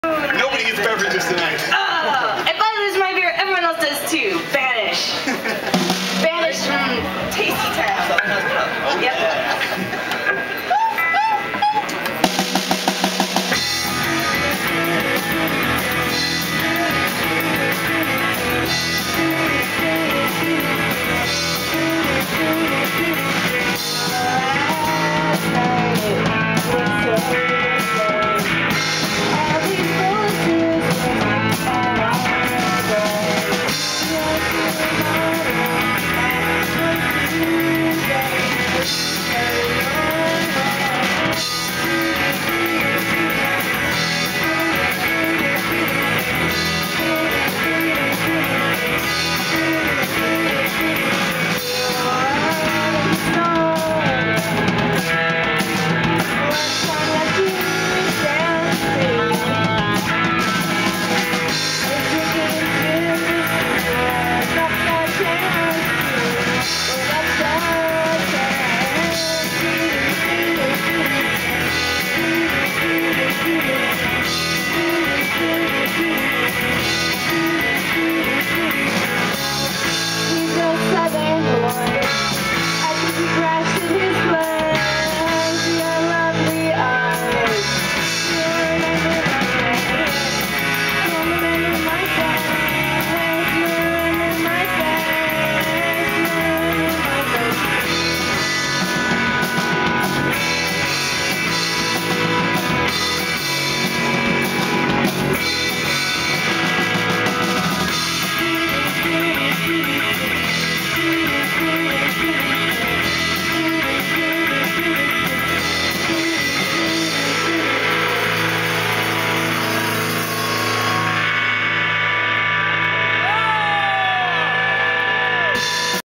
Bunnygrunt at San Francisco Popfest 2009!
St. Louis' Bunnygrunt playing what I'm almost certain is a song from their new record coming out on Happy Happy Birthday to Me this summer. (Please correct me if I'm wrong.)